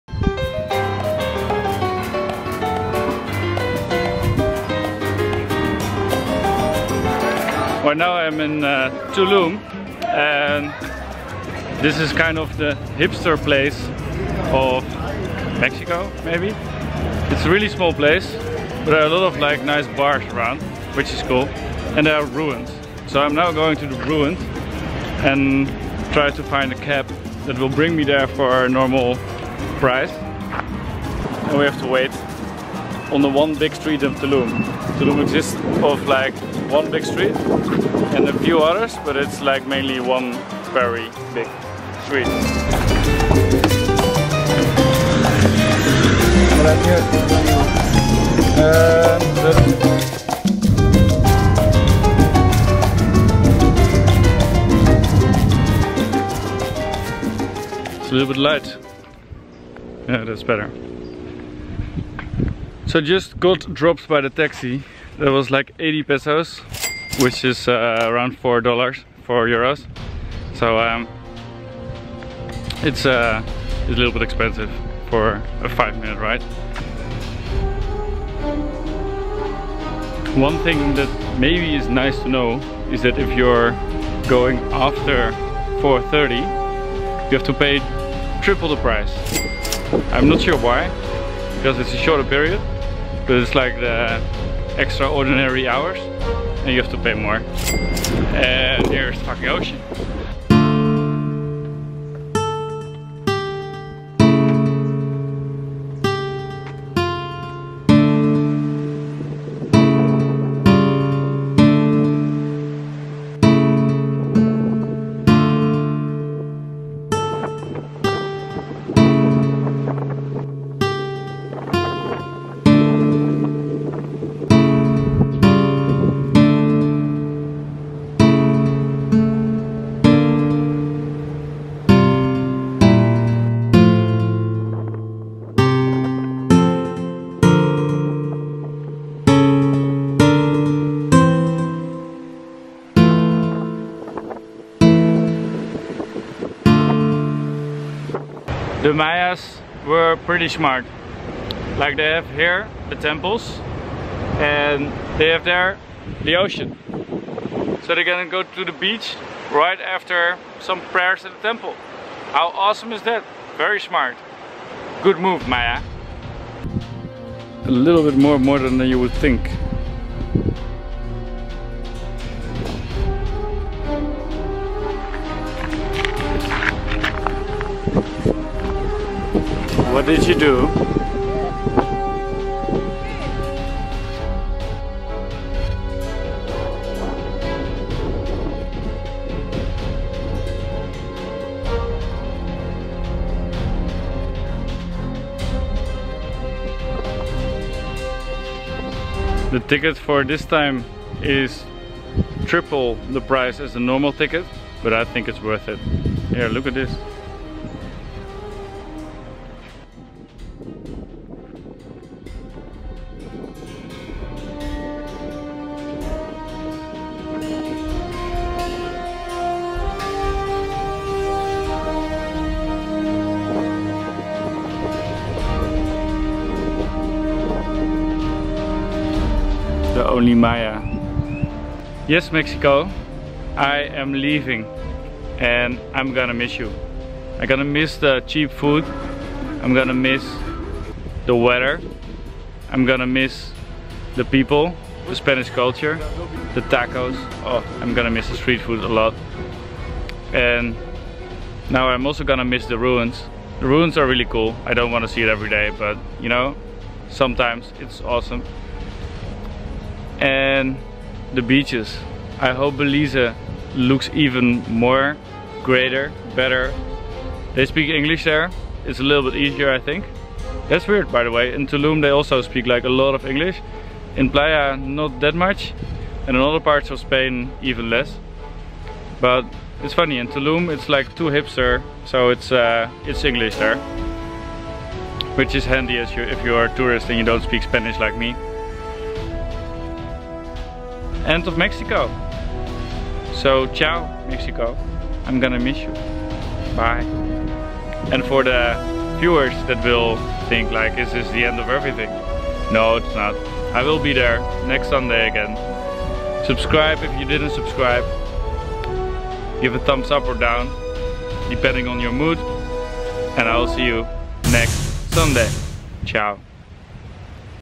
THE LAST MAYA in Tulum, Mexico
The famous stop for the Maya was my last stop in Mexico: Tulum. I'm going to miss Mexico very much. You can see here why. The Maya are still here btw, so it's not really the last one. Enjoy lovely person!

GEAR
PowerShot G7 X Mark II
Joby GorillaPod
Zoom H1 Handy Recorder
MacBook 12" with Final Cut Pro